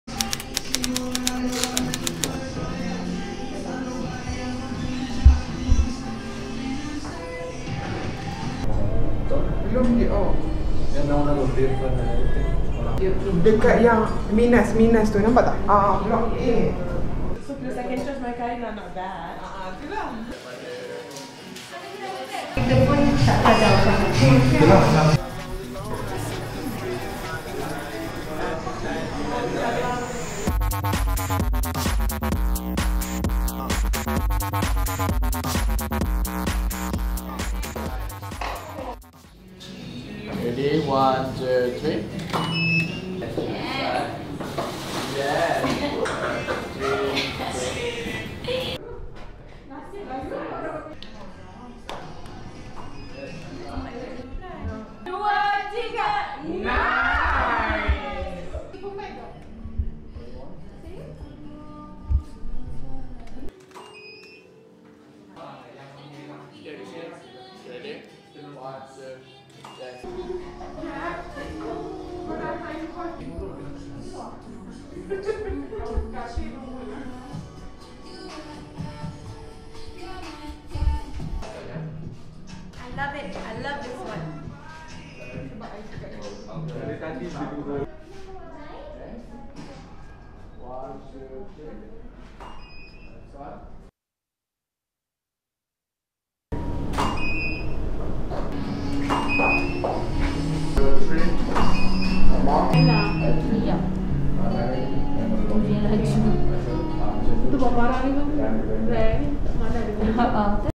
MINAZBTS | Behind The Scene Campaign shoot with Nora Danish
Throwback to our last campaign shoot with Nora. Lovely!